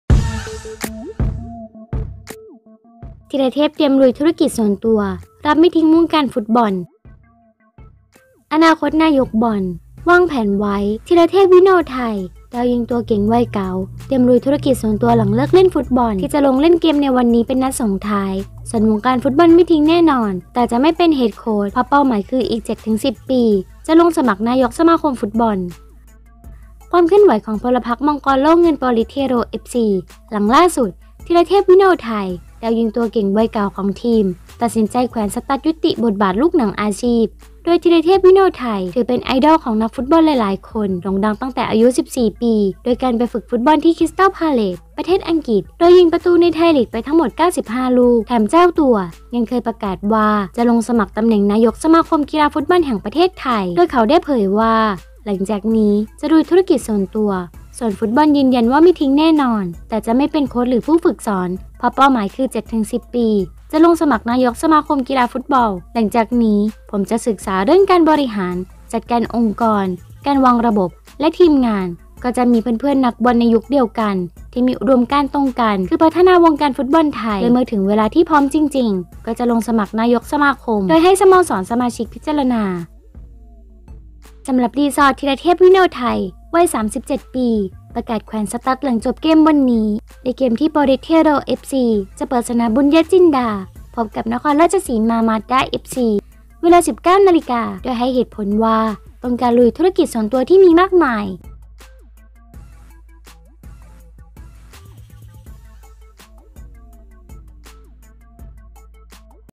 “ธีรเทพ” เตรียมลุยธุรกิจส่วนตัว รับไม่ทิ้งวงการฟุตบอลอนาคตนายกบอล
วางแผนไว้! ธีรเทพ วิโนทัย ดาวยิงตัวเก่งวัยเก๋า เตรียมลุยธุรกิจส่วนตัวหลังเลิกเล่นฟุตบอลที่จะลงเล่นเกมในวันนี้เป็นนัดส่งท้าย ส่วนวงการฟุตบอลไม่ทิ้งแน่นอน แต่จะไม่เป็นเฮดโค้ชเพราะเป้าหมายคืออีก 7-10 ปี จะลงสมัครนายกสมาคมฟุตบอลฯ